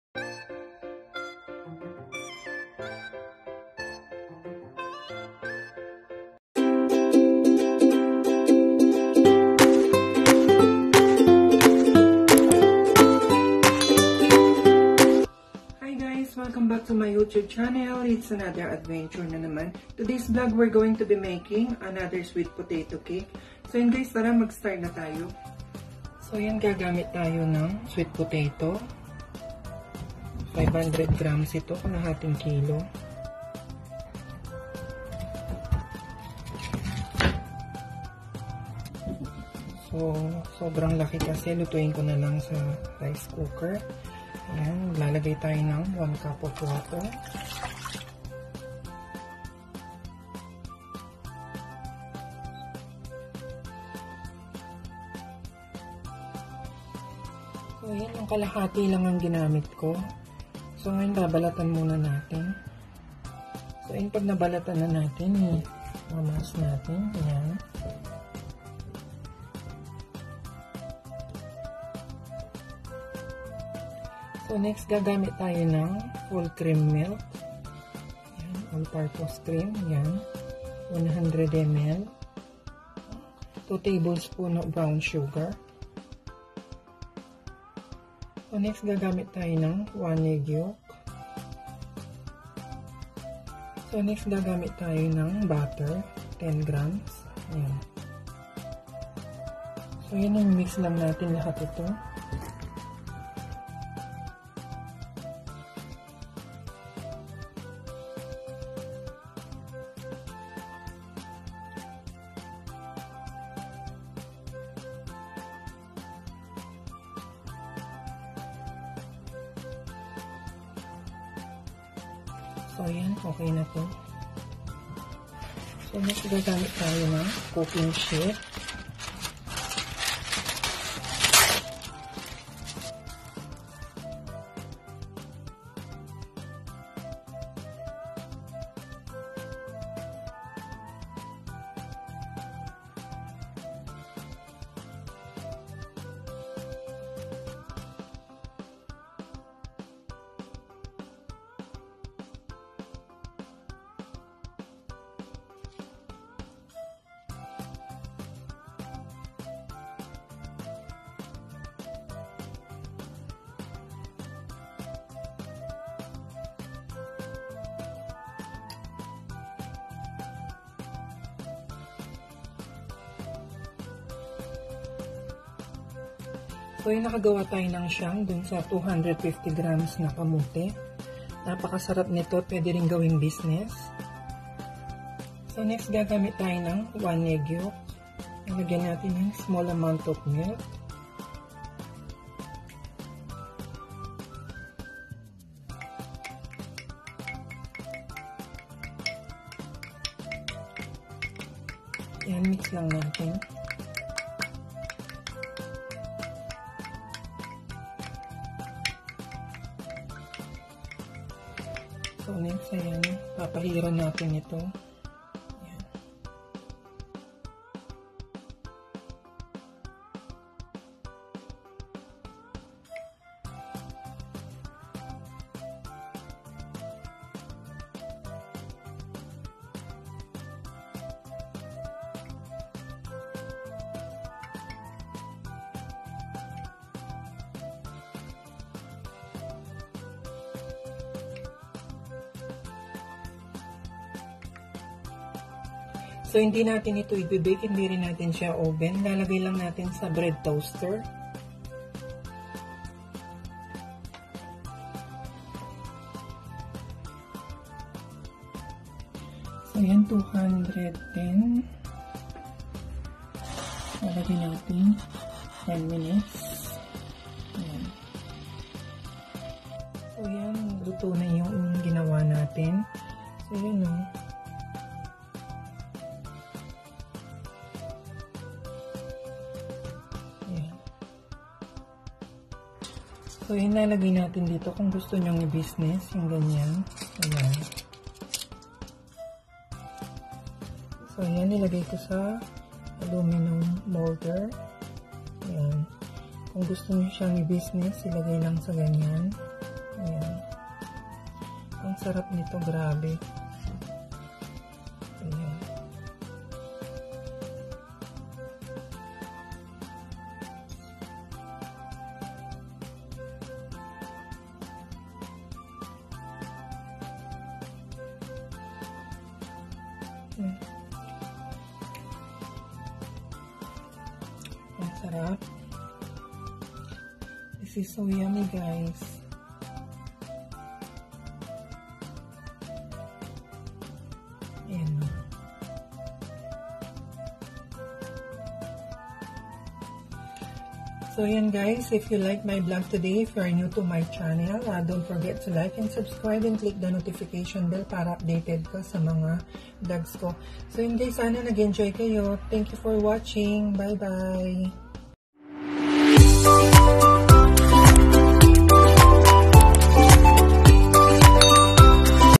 CREAMY SWEET POTATO CAKE !! クリーミーなサツマイモケーキ !! Quick and Easy to Cook !! No Oven, No Bake !! Yummy
INGREDIENTS

Creamy Sweet Potato Cake

Sweet Potato 250 grams
サツマイモ250グラム

All Purpose Cream 100ml
生クリーム100ml

Granulated Brown Sugar 2 tbsp
グラニュー糖大さじ2

バター10グラム

卵黄1個

Milk 1tsp
ミルク小さじ1

Cook
Bread Toaster 210'c 10 minutes
パントースター210c10分